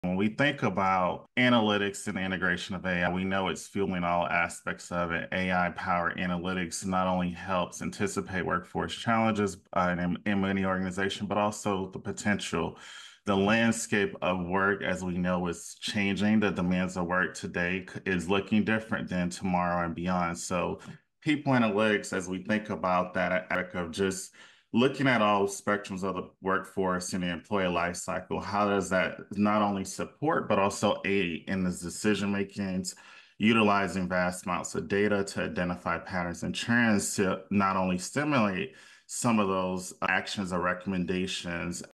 Prioritizing humanity in the age of AI
At PEX Network's All Access: Digital Transformation in HR webinar series, Tyrone Smith Jr., program faculty member /adjunct associate professor at University of Southern California (USC), shared how to ensure people remain at the core as advanced technology like AI becomes more prominent.

Have a question? We are here to help.